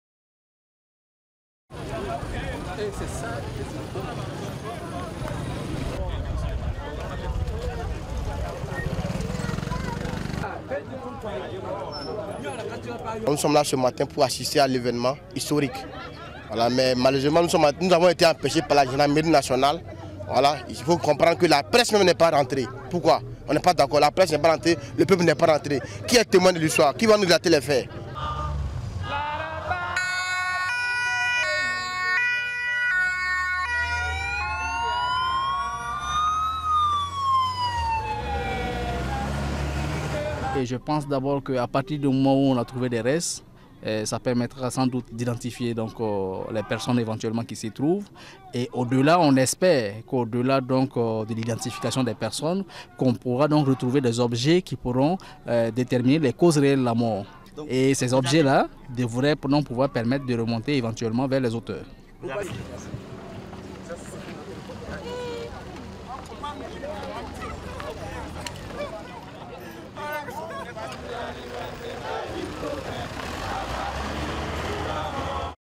Burkina: exhumation de Sankara et de ses compagnons
L'exhumation du corps de l'ex-président burkinabé Thomas Sankara, assassiné en 1987 lors d'un coup d’État, commence dans un cimetière de Ouagadougou où les tombes de deux de ses compagnons ont déjà été ouvertes ce lundi.